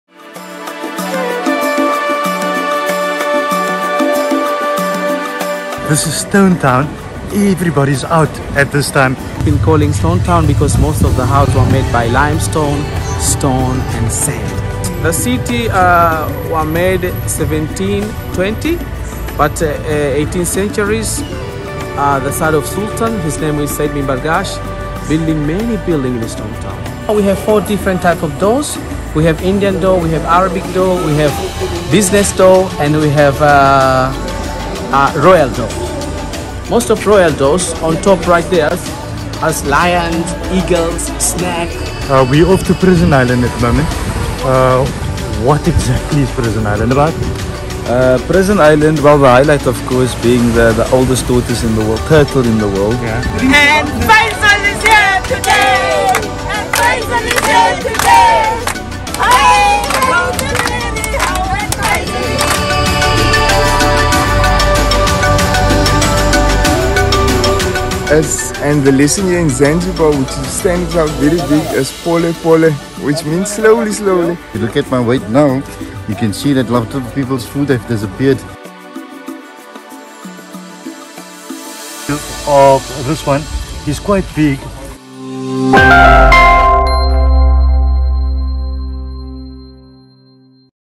197 year old Tortoise on Prison Island | Faizal Sayed EXTRA
A visit to Stone Town and Prison Island to see a 197 year old Giant Tortoise.